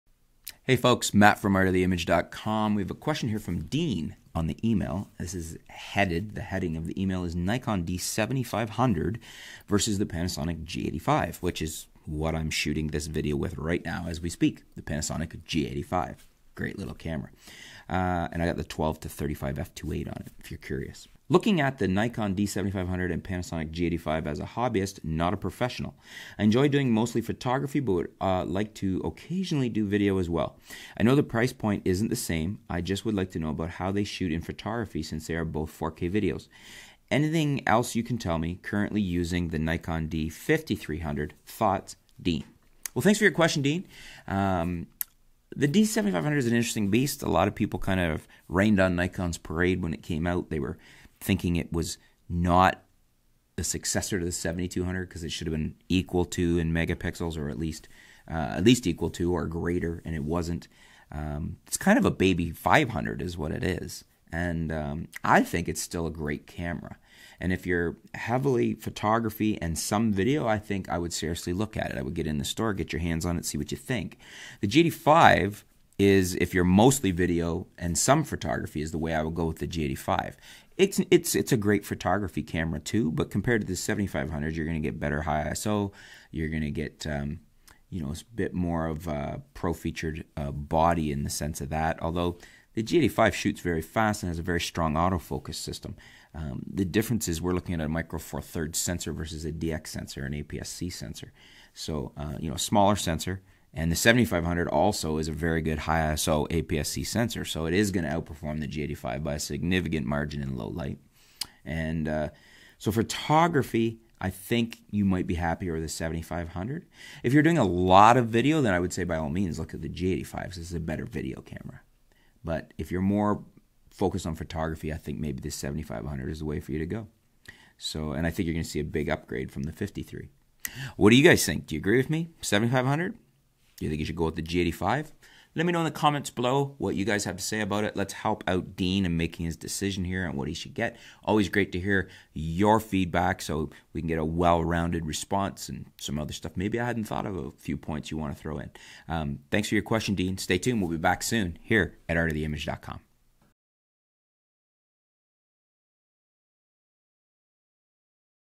Nikon D7500 vs Panasonic G85 - Which is the BETTER Photography Camera Since Both Shoot 4K?
Panasonic LUMIX G85 4K Mirrorless Interchangeable Lens Camera on Amazon

Columbia Men’s Steens Mountain Full Zip Fleece 2.0